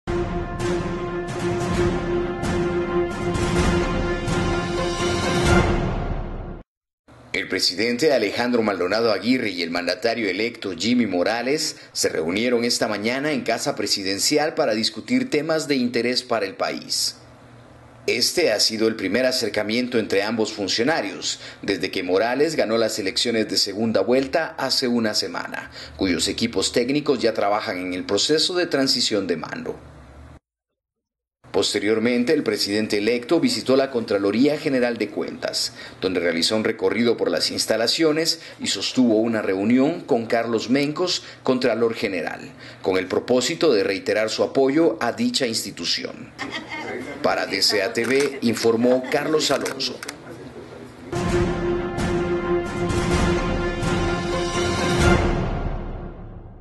Maldonado Aguirre y Morales sostienen reunión
El presidente Alejandro Aguirre y el mandatario electo Jimmy Morales se reunen esta mañana en Casa Presidencial para discutir temas de interés para el país.